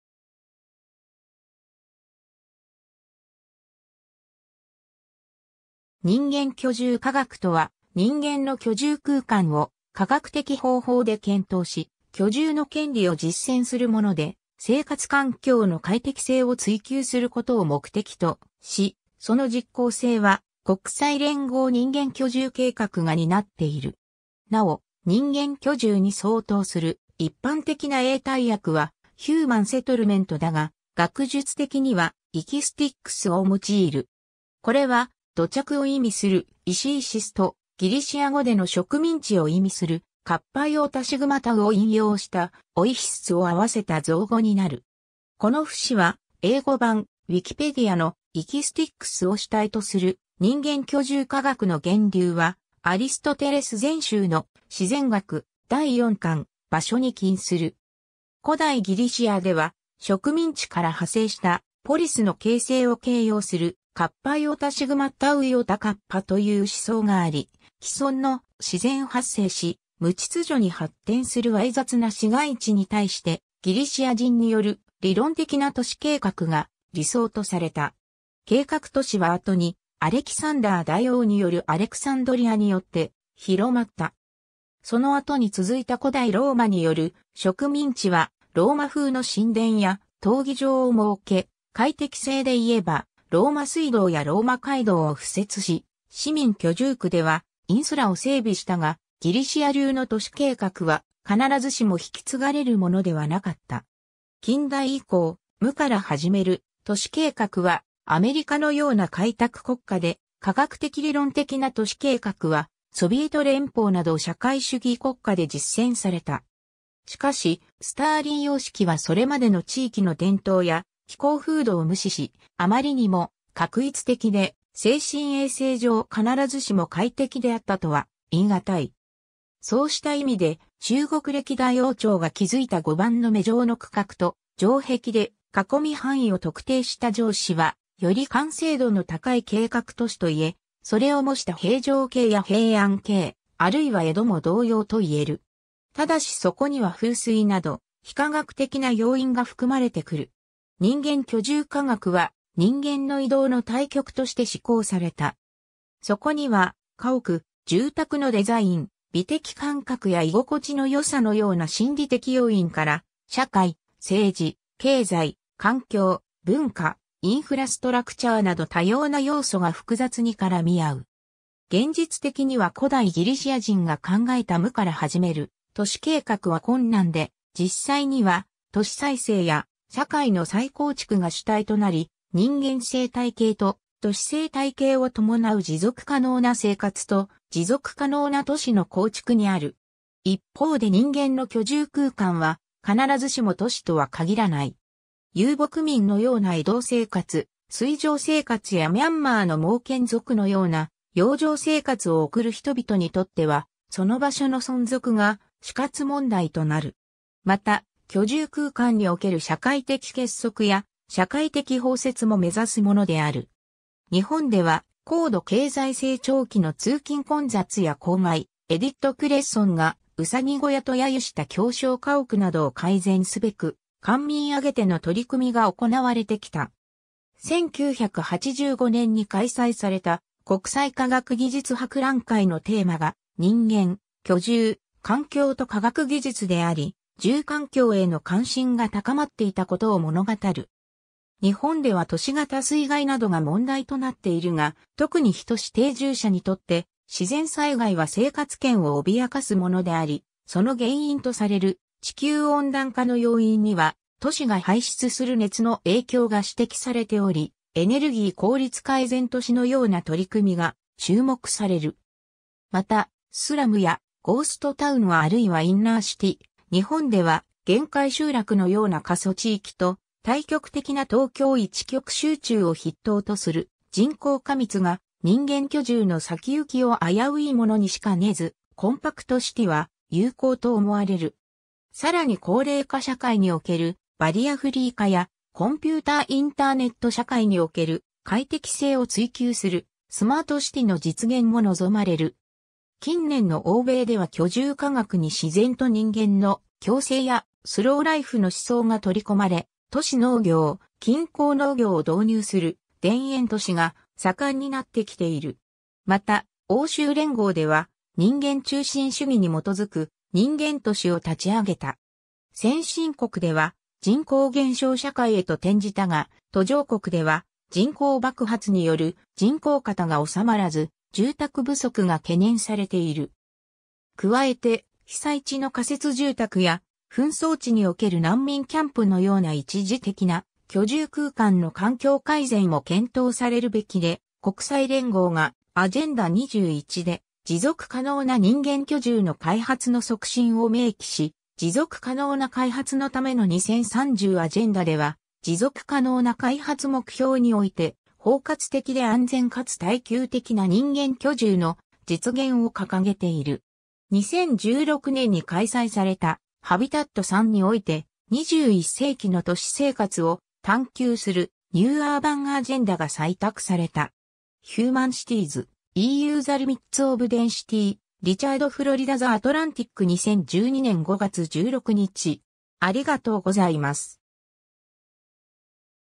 人間居住科学
人間居住科学（にんげんきょじゅうかがく）とは、人間の居住空間を科学的方法で検討し、居住の権利を実践するもので、生活環境の快適性を追求することを目的とし、その実行性は国際連合人間居住計画が担っている。
 なお、「人間居住」に相当する一般的な英対訳は「Human settlement」だが、学術的には「Ekistics」を用いる。
これは「土着」を意味する「Ecesis」とギリシア語での植民地を意味する「οἰκιστής」を引用した「Oikistes」を合わせた造語になる。
 この節は英語版Wikipediaの「Ekistics」を主体とする 人間居住科学の源流はアリストテレス全集の『自然学』第4巻「場所」に起因する。
古代ギリシアでは「植民地(οἰκιστής)」から派生した「ポリス（都市）の形成」を形容する「οἰκιστικός」という思想があり、既存の「自然発生し無秩序に発展する猥雑な市街地」に対してギリシア人による「理論的な都市計画」が理想とされた。
計画都市は後にアレキサンダー大王によるアレクサンドリアによって広まった。
 その後に続いた古代ローマによる植民地（ローマ都市）はローマ風の神殿や闘技場を設け、快適性でいえばローマ水道やローマ街道を敷設し、市民居住区ではインスラを整備したが、ギリシア流の都市計画は必ずしも引き継がれるものではなかった。
 近代以降、無から始める都市計画はアメリカのような開拓国家で、科学的理論的な都市計画はソビエト連邦など社会主義国家で実践された。
しかし、スターリン様式はそれまでの地域の伝統や気候風土を無視し、あまりにも画一的で精神衛生上必ずしも快適であったとは言いがたい。
 そうした意味で中国歴代王朝が築いた碁盤の目状の区画と城壁で囲み範囲を特定した城市は、より完成度の高い計画都市といえ、それを模した平城京や平安京、あるいは江戸も同様といえる。
但しそこには風水など非科学的な要因が含まれてくる。
 　　　＊　　　＊　　　＊ 人間居住科学は人間の移動（マイグレーション）の対局として思考された。
そこには家屋・住宅のデザイン、美的感覚や居心地の良さのような心理的要因から、社会・政治・経済・環境・文化・インフラストラクチャーなど多様な要素が複雑に絡み合う。
 現実的には古代ギリシア人が考えた無から始める都市計画は困難で、実際には都市再生や社会の再構築が主体となり、人間生態系と都市生態系を伴う持続可能な生活と持続可能な都市の構築にある。
 一方で人間の居住空間は必ずしも都市とは限らない。
遊牧民のような移動生活、水上生活やミャンマーのモーケン族のような洋上生活をおくる人々にとっては、その場所の存続が死活問題となる。
 また、居住空間における社会的結束や社会的包摂も目指すものである。
 日本では高度経済成長期の通勤混雑や公害、エディット・クレッソンが「うさぎ小屋（cage a lapins）」と揶揄した狭小家屋などを改善すべく、官民あげての取り組みが行われてきた。
1985年（昭和60年）に開催された国際科学技術博覧会のテーマが「人間・居住・環境と科学技術（Dwellings and Surroundings - Science and Techonology for Man at Home）」であり、住環境への関心が高まっていたことを物語る。
 日本では都市型水害などが問題となっているが、特に非都市定住者にとって自然災害は生活圏を脅かすものであり、その原因とされる地球温暖化の要因には都市が排出する熱の影響が指摘されており、エネルギー効率改善都市のような取り組みが注目される。
また、スラムやゴーストタウンあるいはインナーシティ、日本では限界集落のような過疎地域と対極的な東京一極集中を筆頭とする人口過密が人間居住の先行きを危ういものにしかねず、コンパクトシティは有効と思われる。
さらに高齢化社会におけるバリアフリー化や、コンピューターインターネット社会における快適性を追求するスマートシティの実現も望まれる。
 近年の欧米では居住科学に自然と人間の共生やスローライフの思想が取り込まれ、都市農業・近郊農業を導入する田園都市が盛んになってきている。
また、欧州連合（EU）では人間中心主義（ヒューマニズム）に基づく人間都市（ヒューマンシティ）を立ち上げた。
 先進国では人口減少社会へと転じたが、途上国では人口爆発による人口過多が収まらず、住宅不足が懸念されている。
加えて被...